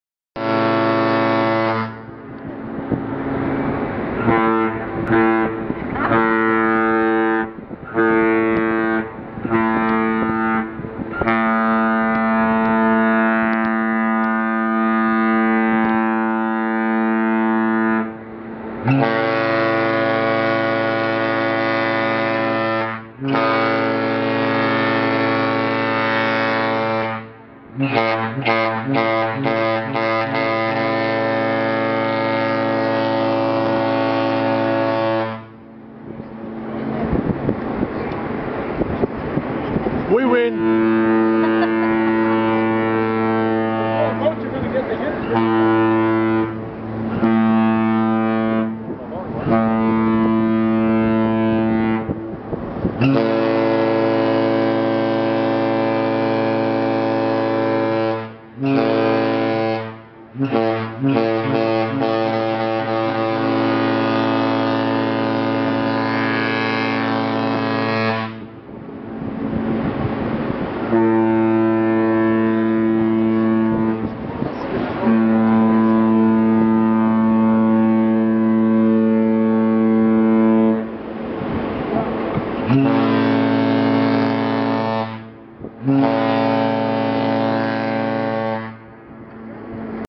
Battle of the Ship's Horns
Two Holland American ships in Cabo San Lucas, Mexico battle it out as they leave port on 29th April 2011